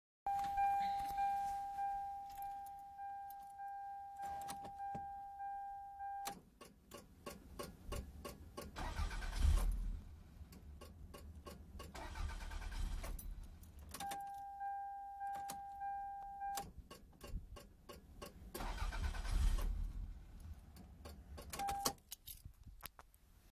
Beams redtop + 1zz-fe ecu vid1
First attempt to alter wiring to use a 1zz-fe power fc to run the redtop 3s-ge engine, work in progress :P